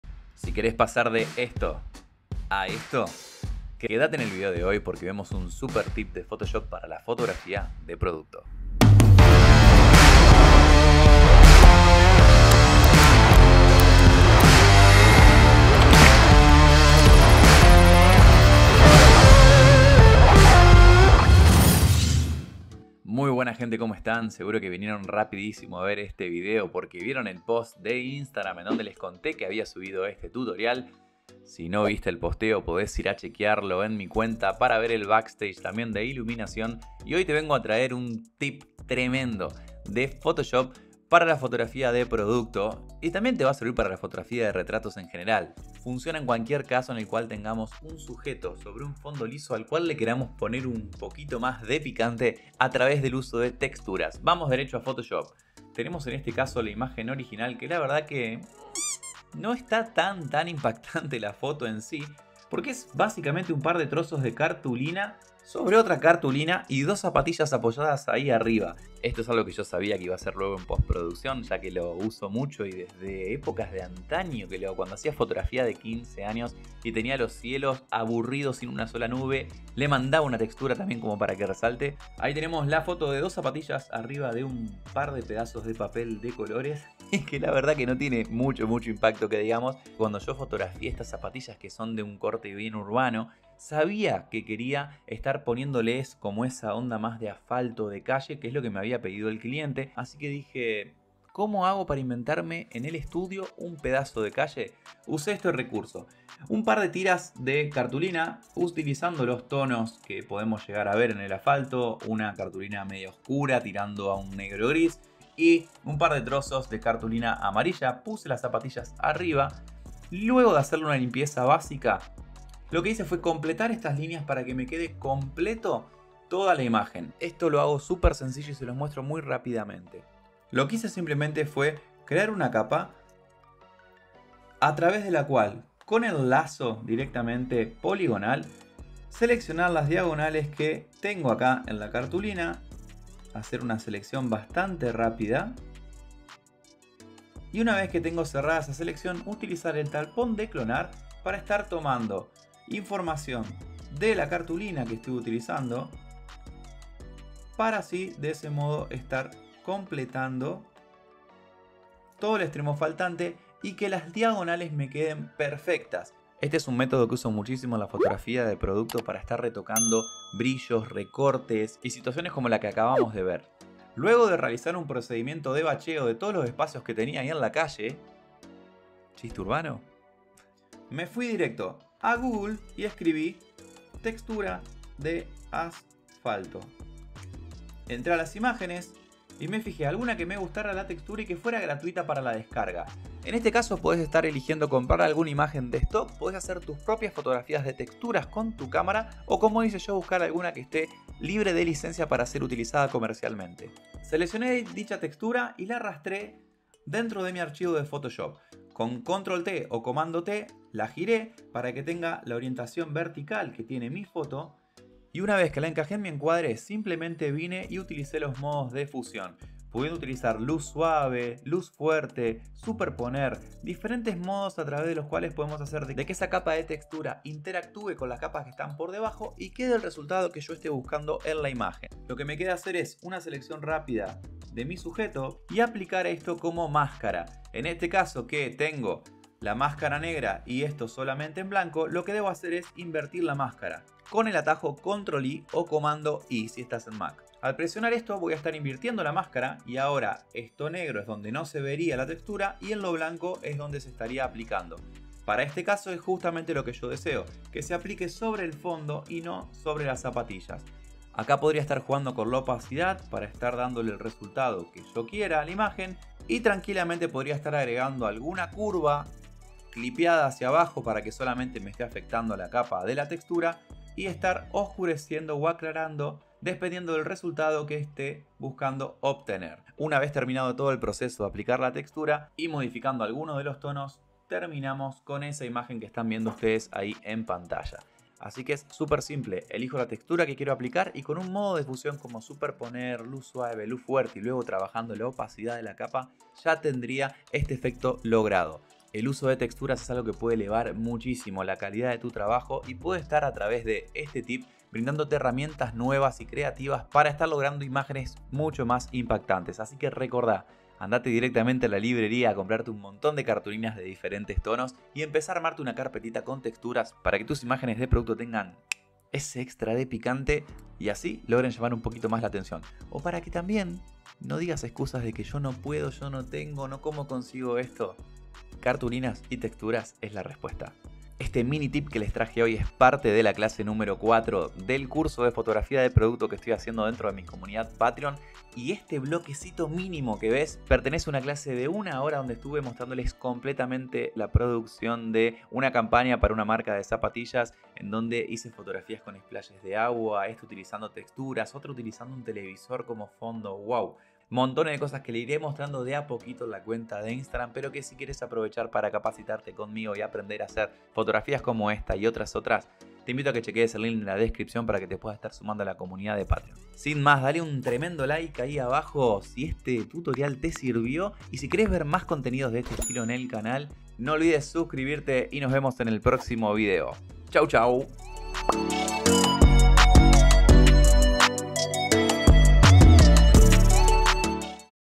Como usar TEXTURAS en Fotografia de Producto | Tutorial de Photoshop
Super Truco Tutorial de Photoshop sobre Como usar texturas para elevar la calidad de tus fotografias de producto y asi crear cada dia mejores fotografias y mas creativas. Paso a paso de como arme la foto y mi prensamiento en preproduccion para lograr una correcta post produccion.

Si estas en ARGENTINA y queres abonar en pesos enviame un mensaje privado por instagram para que te pase el dato correspondiente: